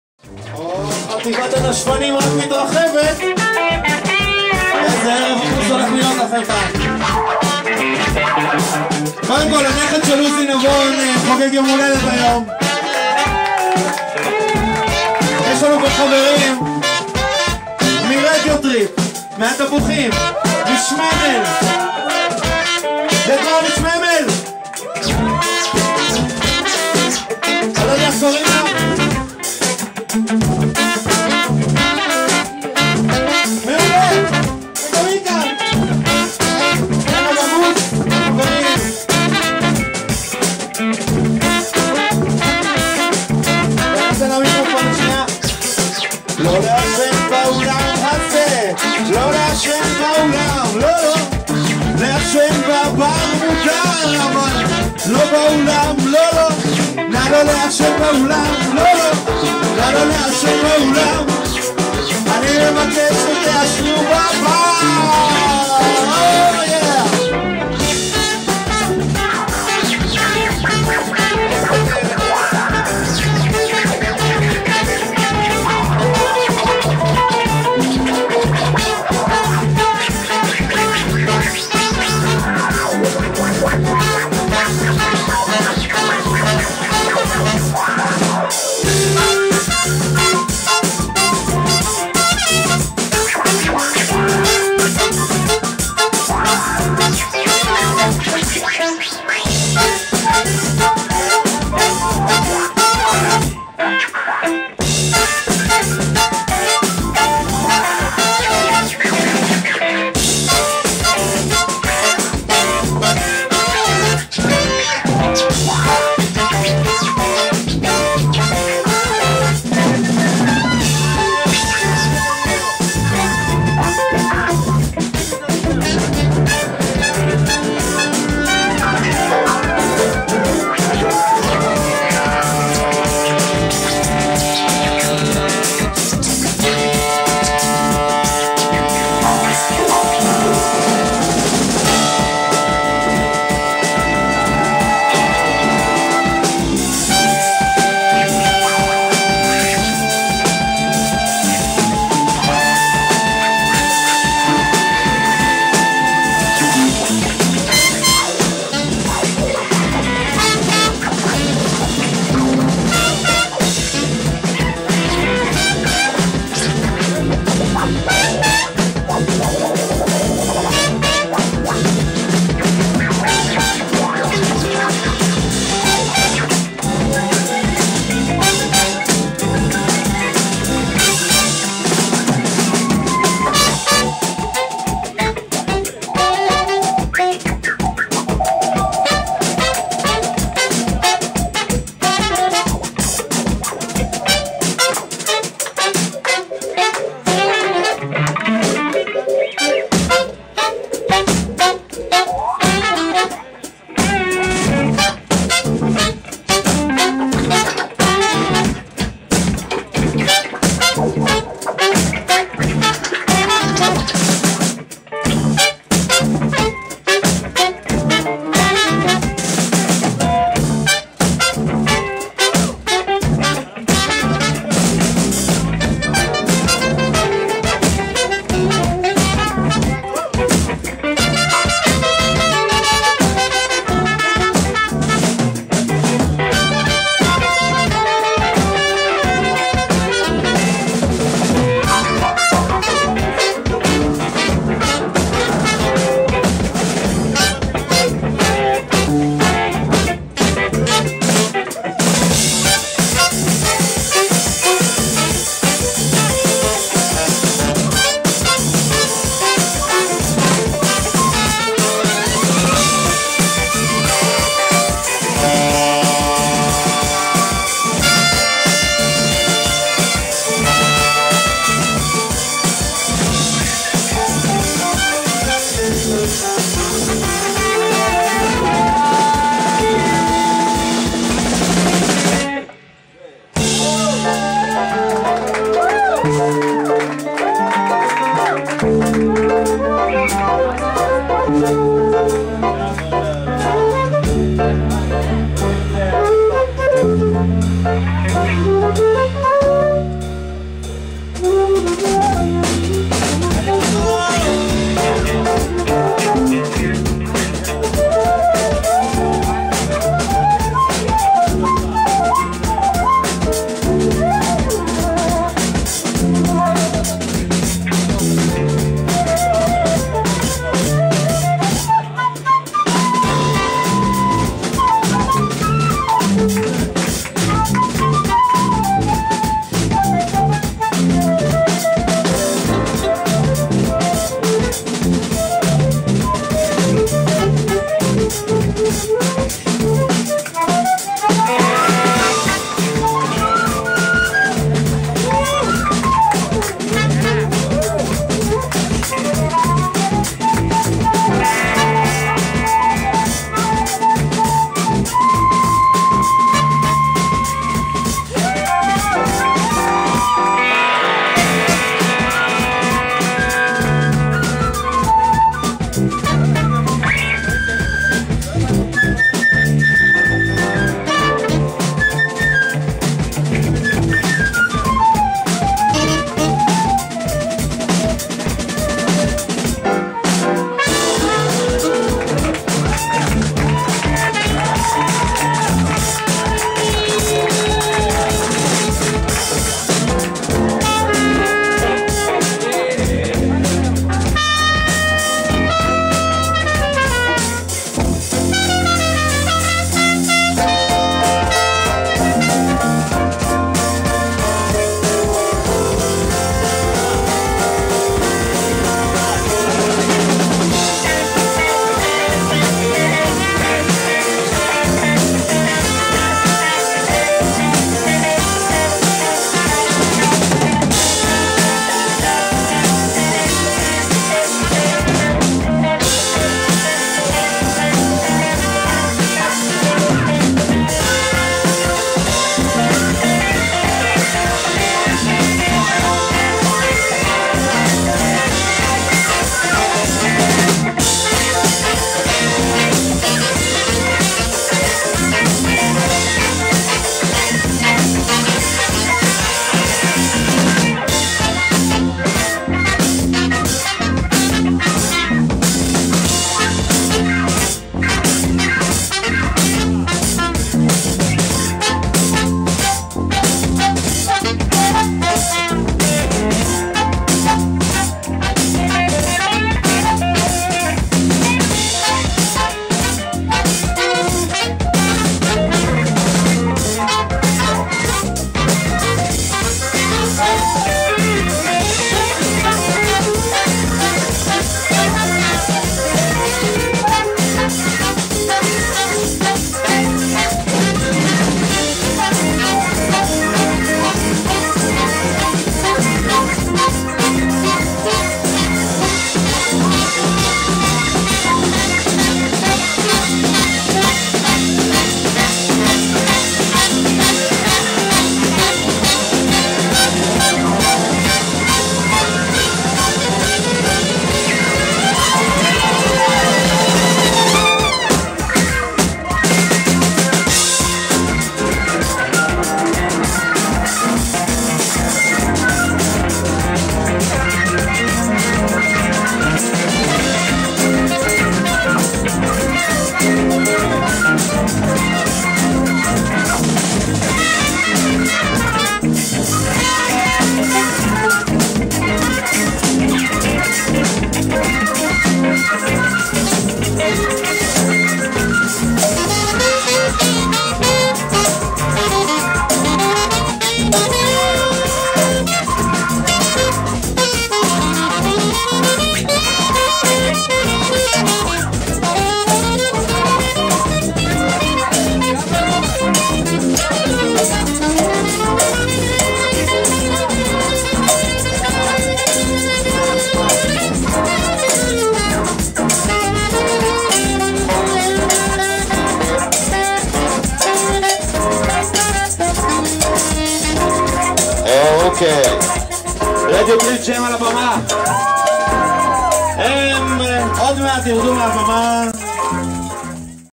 לכלוך באוזן #18 חלק שני
לאון נפולאון פלדמן בשיתוף רדיו טריפ מציגים raw sounds נא באוזן

ערב על טהרת הגרוב והליבלוב, הבלוז והשמוז הפאנק והסקאנק
נא באוזן | שישי | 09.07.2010 | החל מ 22:30 | אוזןבר | עשרים שקלים בלבד

Radio Trip All-stars Jam
סקולמאסטר ומיקסמנסטר מארחים חברים מהתפוחים, האחים רמירז, עוזי נבון ומכרים, שממל ועוד לג'ם היסטורי והיסטרי

TREE
רביעיית בלוז רוק פסיכדלית ומשובחה במופע מעבר לתודעה

(רועי ריק ועידן רבינוביץ' (הקולקטיב
מפוחיתן הבלוז הטוב בארץ פוגש את אביר הנשמה במופע של חומרים מקוריים ואילתור

עוזי פיינרמן
אגדת הגיטרה, עוזי בלוזי רמירז במופע הוואן מן בנד המוצלח בלבנט

So Long Suckers
רביעיית הרוקבילי ההרמונית והמפתה בעיר

3421
דואו שעשועי הסינתי מבית היוצר של הברוקן פינגז

Left
צמד פוסט רוק שאת אלבום הבכורה שלו הפיקו יחד עוזי פיינרמן ואורי ורטהיים במופע מסמר שיער

ירון בן עמי
מהפכן הבלוז מארץ כנען